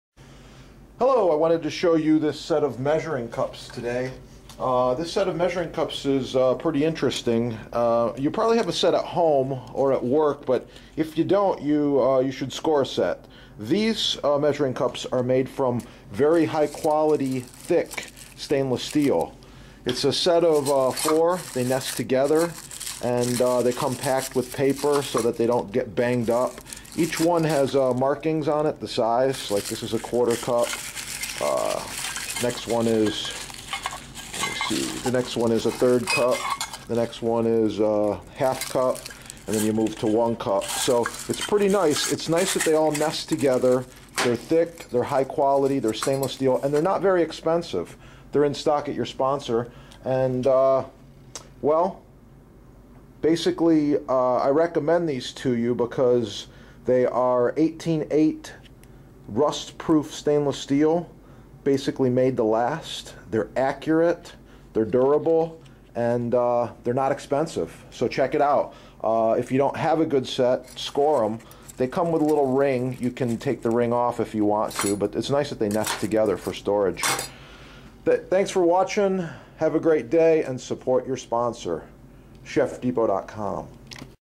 measuring cups review
Chefs review of a quality measuring cup set and and Vanilla beans Chef Tools.